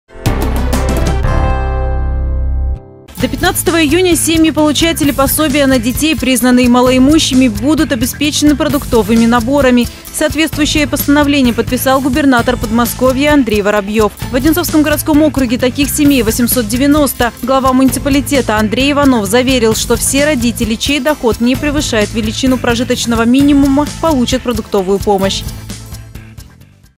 Поддержка семей
До 15 июня семьи-получатели пособия на детей, признанные малоимущими, будут обеспечены продуктовыми наборами. Соответствующее постановление подписал губернатор Подмосковья Андрей Воробьёв. В Одинцовском городском округе таких семей 890.